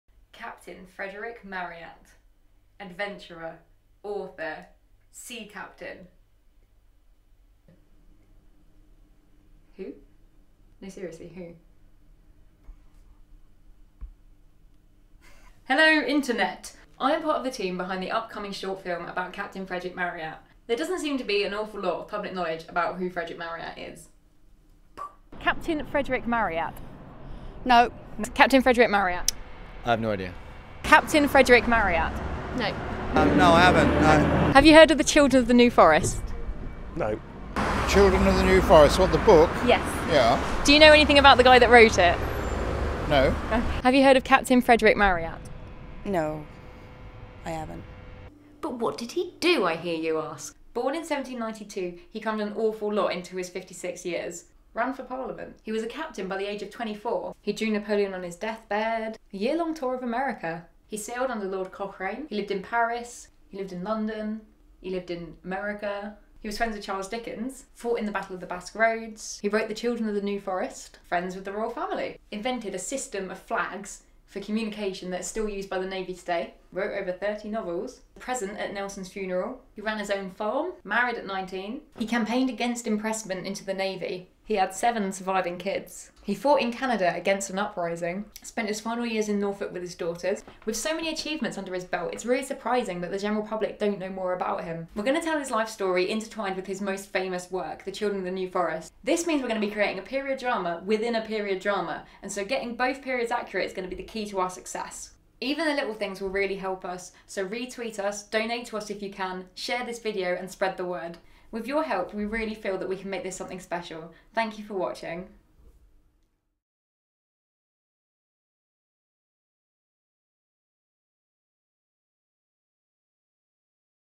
Captain Frederick Marryat, an Introduction
Katherine Grant, part of the 'Marryat' team, leads you lightheartedly into his life.